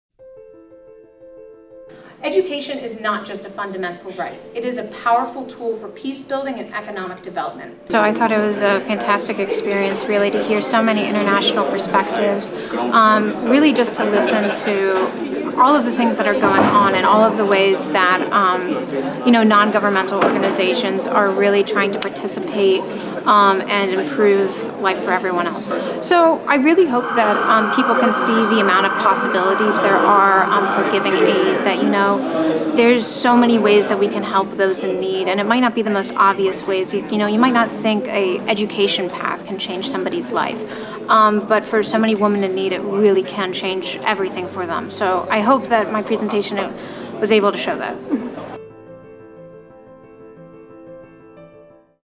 SDGs Roundtable Discussion: Civil Society Best Practices and Initiatives for the SDGs
We are proud to share a glimpse of this year's SDGs Roundtable Discussion, Round 2: Civil Society Best Practices and Initiatives for the SDGs that our Women's Empowerment Program Coordinator, Sarah Elsakhawy, was part of hosted by the Journalists and Writers Foundation (JWF).

She shared some incredible insights with the panel about our Women's Empowerment program and how important our work is to the lives of hundreds of women and girls worldwide.